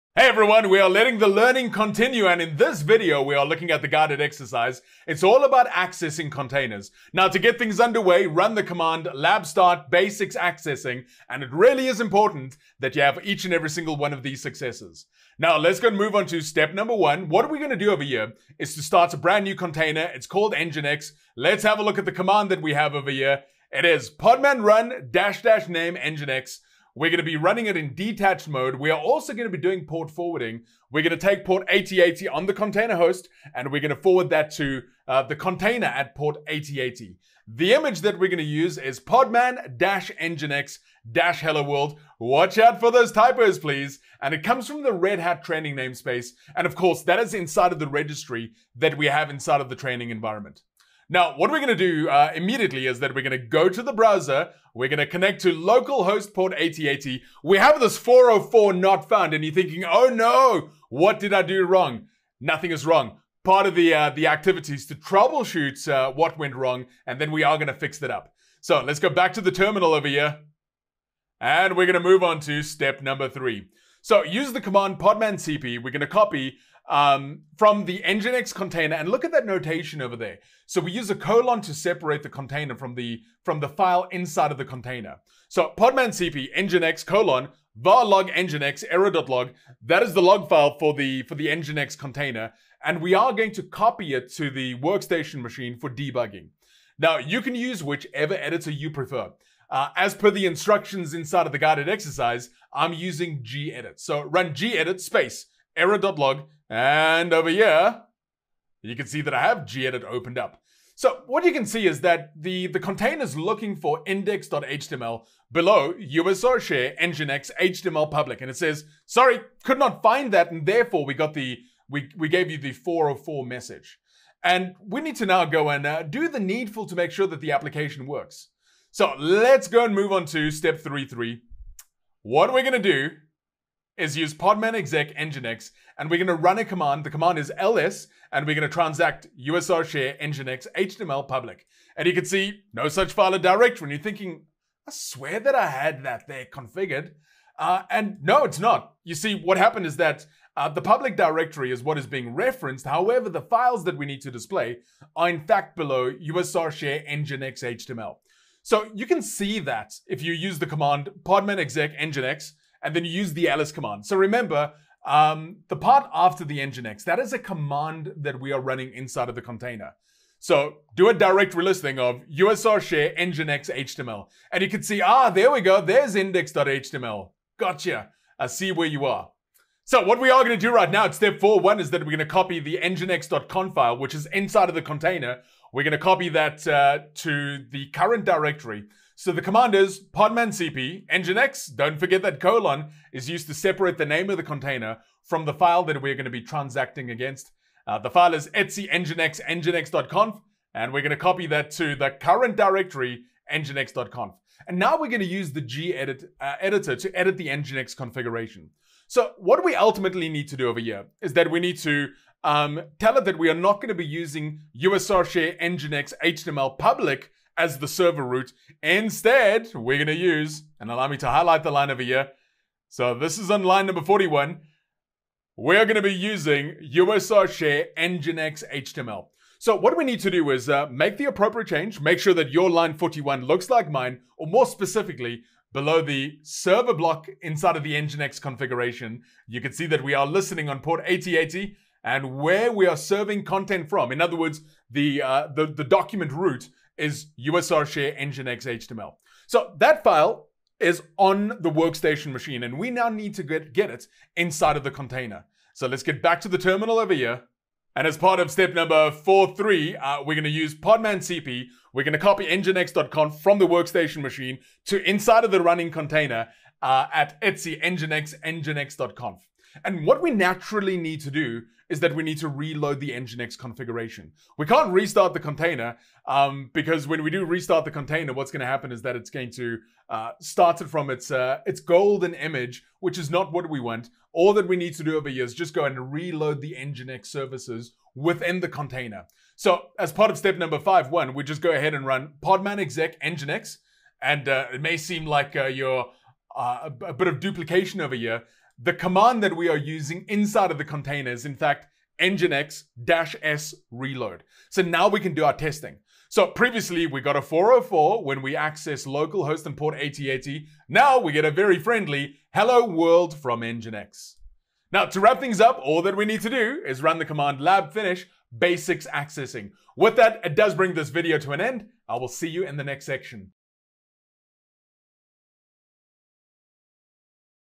DO188 Red Hat OpenShift Development I: Guided Exercise: Accessing Containers
Use the podman exec and podman cp commands to debug and correct container configuration.

Outcomes

You should be able to:

Use the podman exec to execute commands inside of a container.

Use the podman cp command to copy files from and into a container.

As the student user on the workstation machine, use the lab command to prepare your system for this exercise